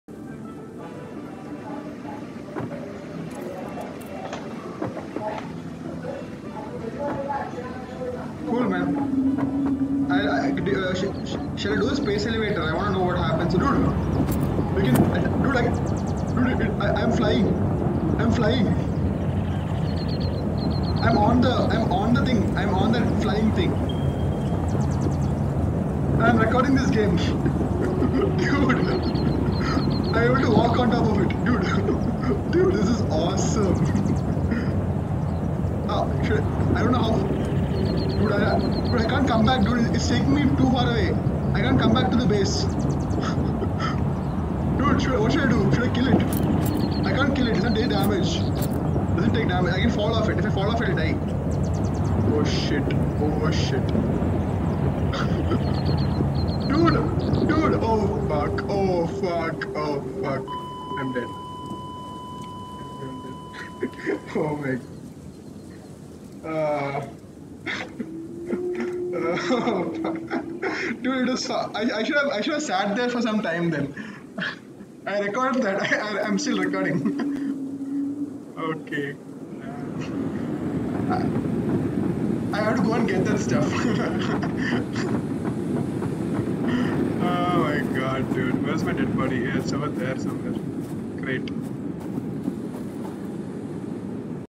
The Gaint Flying Mantas swept me off!
There I was looking over my base when this Gaint Flying Mantas swept me off my feet and carried me with it. I could not enjoy this ride, but I will sure to try it again.